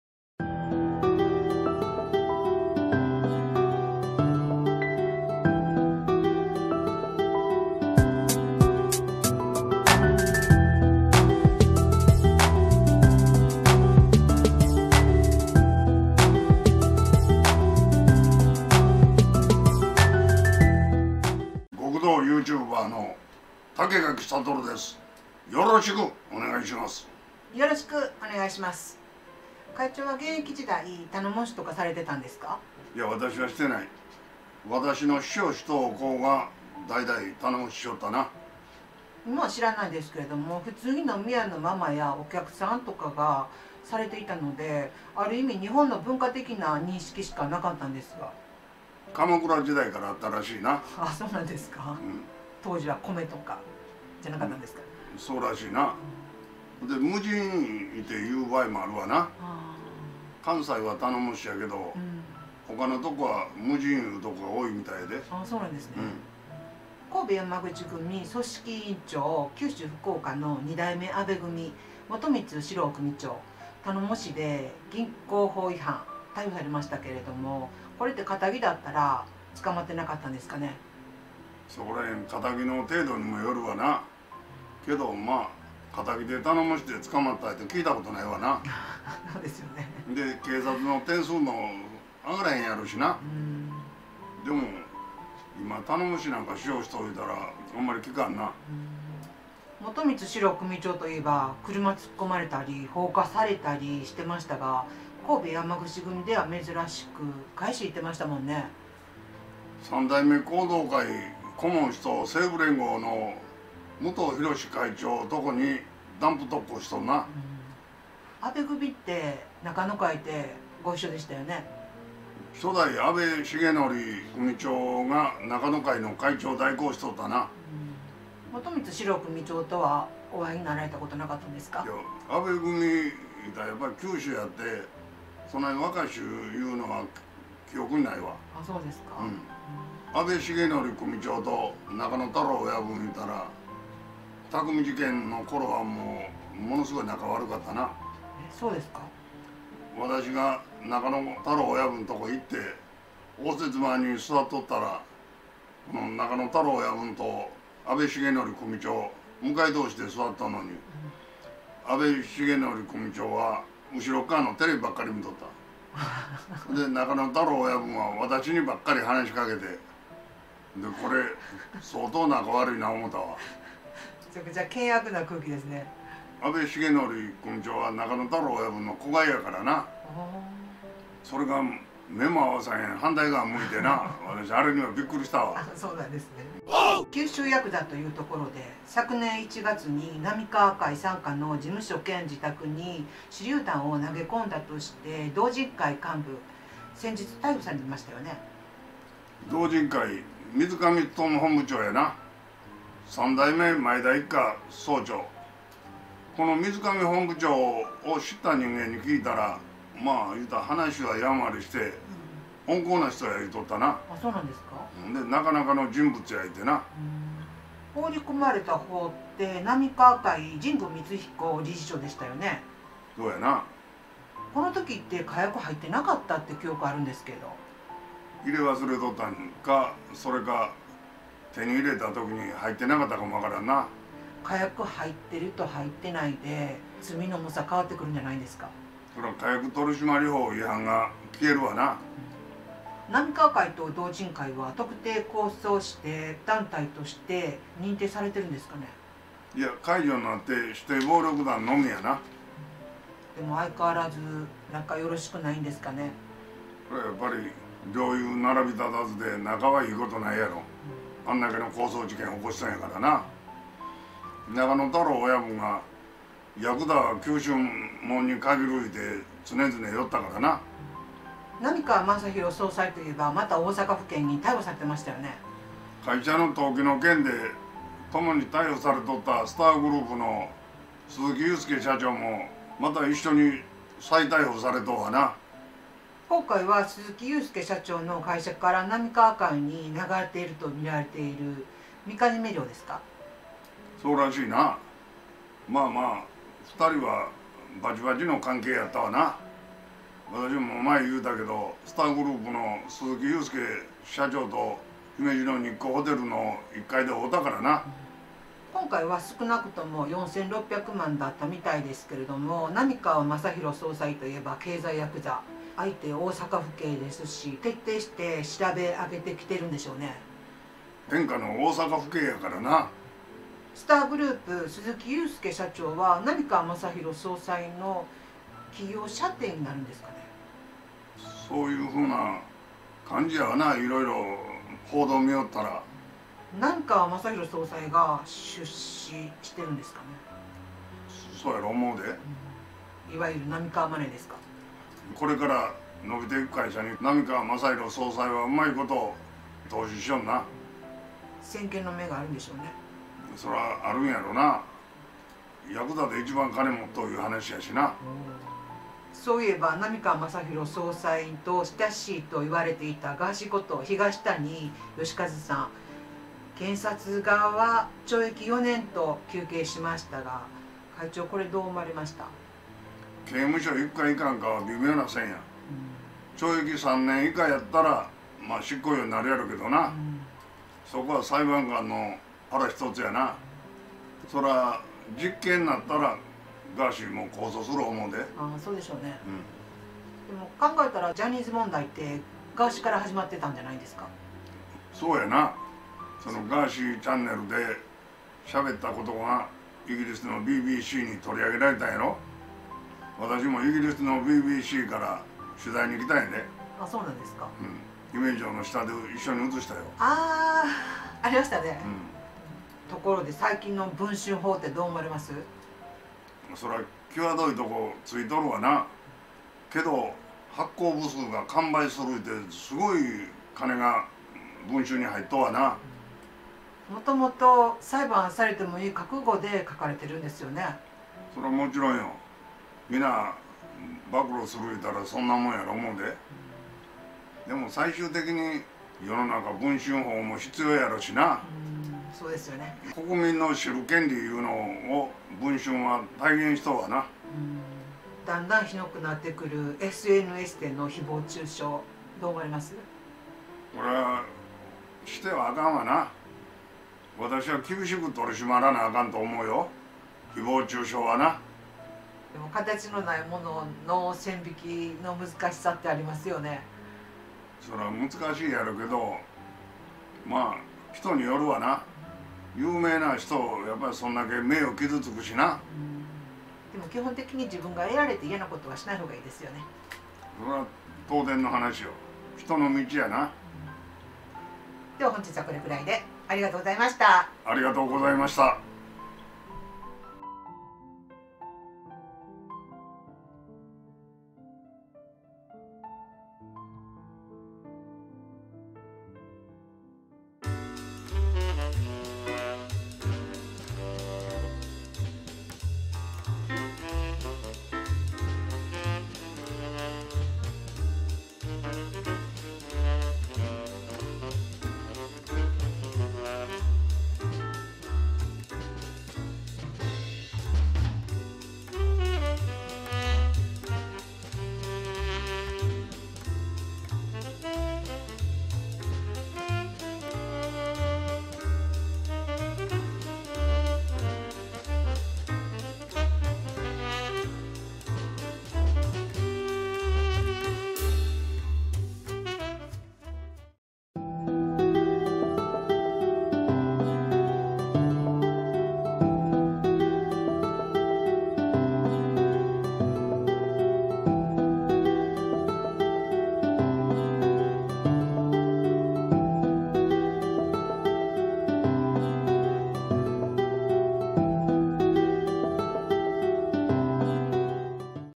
道仁会 水上強本部長 浪川会 神宮光彦理事長事務所に手榴弾を投げ込み逮捕 風俗店スターグループとの蜜月 大阪府警に徹底マークされた浪川政浩総裁 再逮捕
【兄弟チャンネル】

【ファミリーチャンネル】